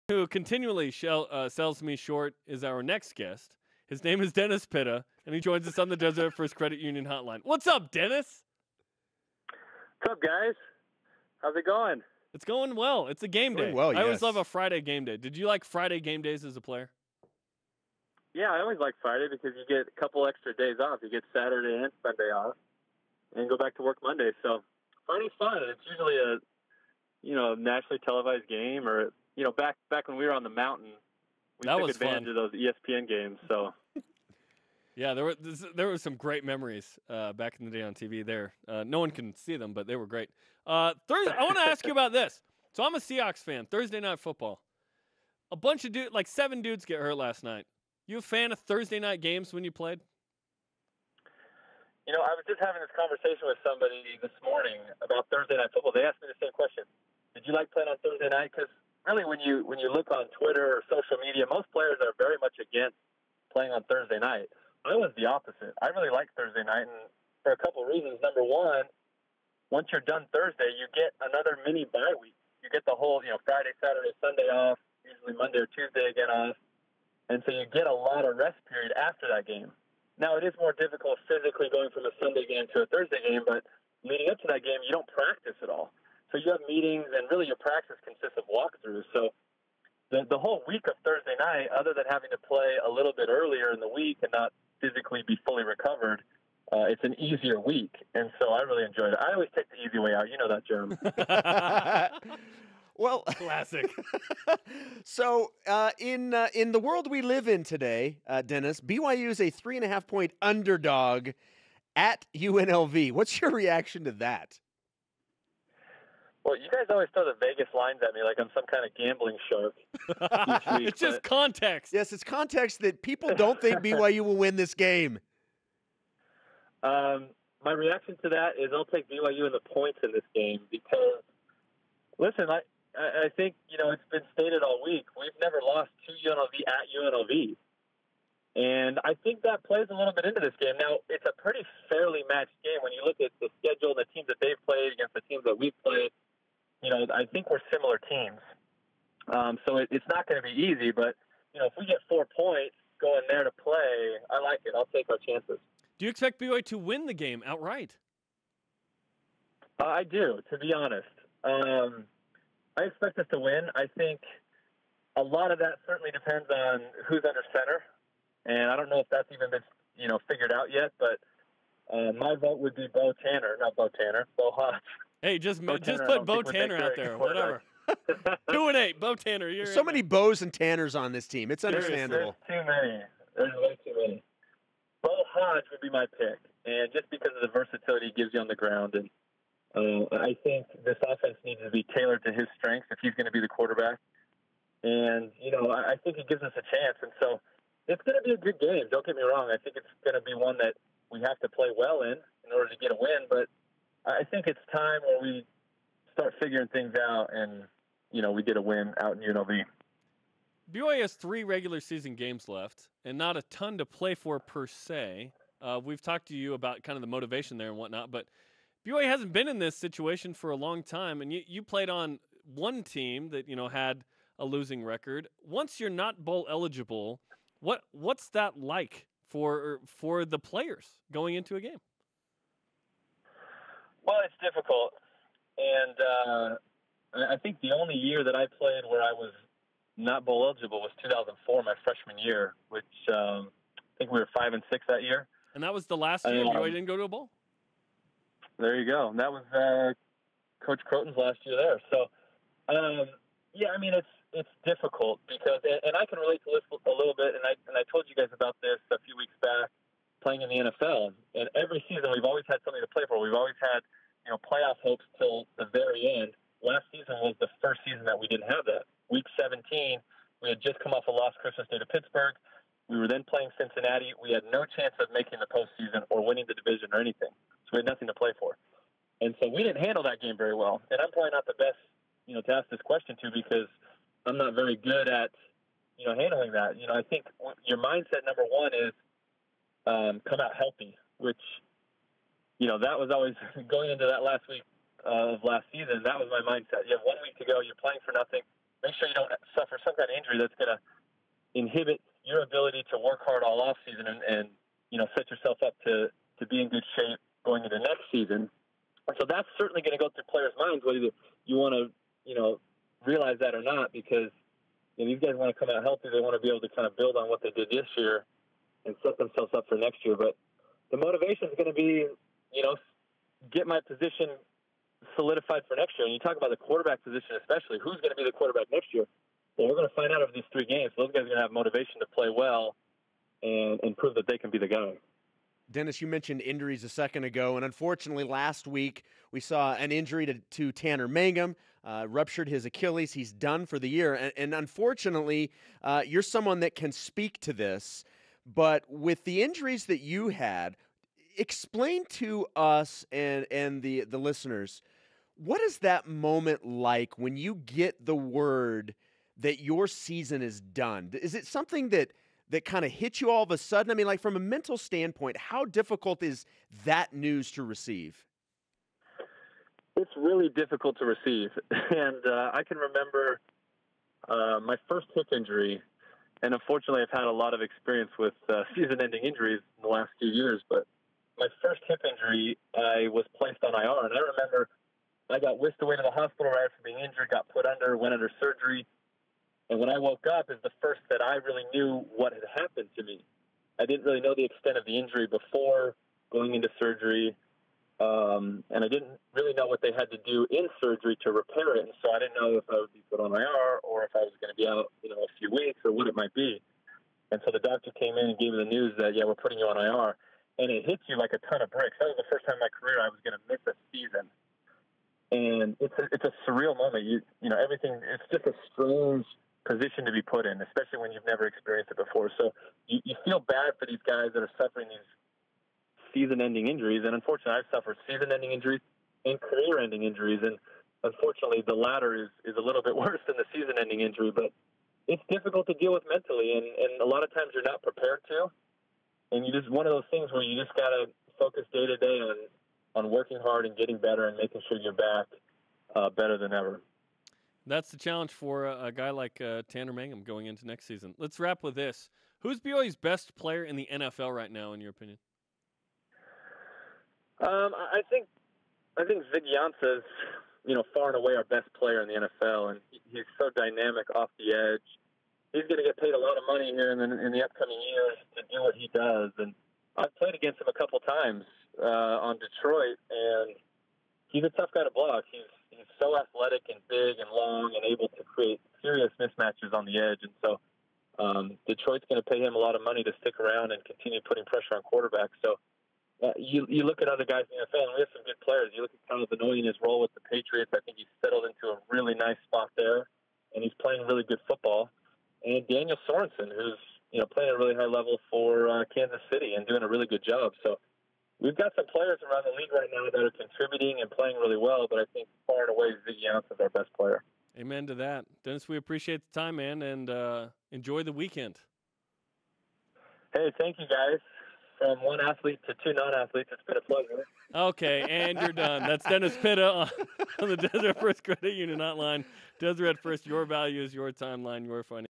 BYUSN - Ravens Radio Analyst Dennis Pitta - 11.10.17
Jarom Jordan's favorite frenemy Ravens Radio Analyst Dennis Pitta joined BYUSN. He discussed the BYU Football match-up vs UNLV plus he analyses who he thinks should be starting QB tonight.